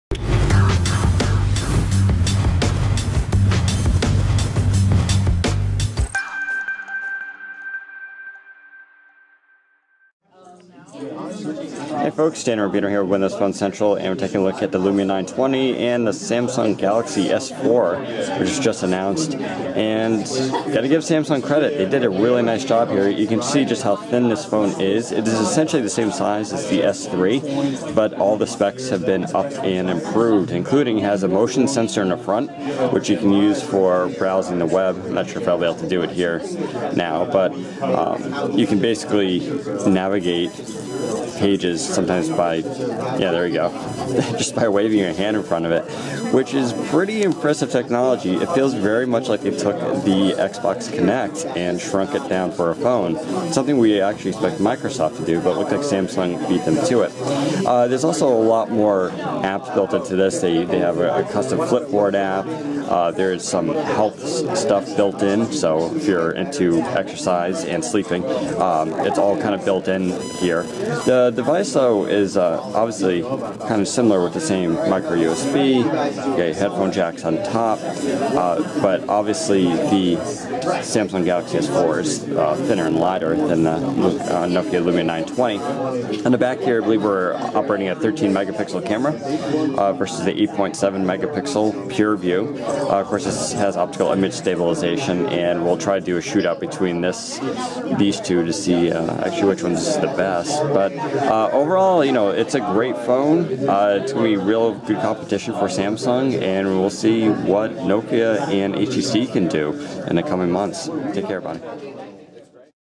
Samsung Galaxy S4 versus Nokia Lumia 920
We briefly compare the just announced Samsung Galaxy S4 and the Nokia Lumia 920.